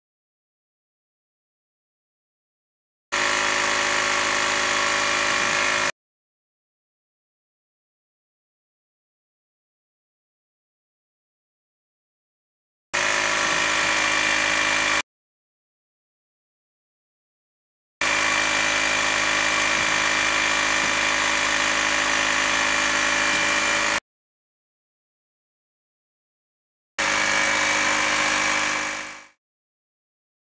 Arduino-Controlled Trash Compactor
This device was created by Boston University students William Holden, Irena Roggeveen, and Megan Stewart as their Introduction to Engineering Design final project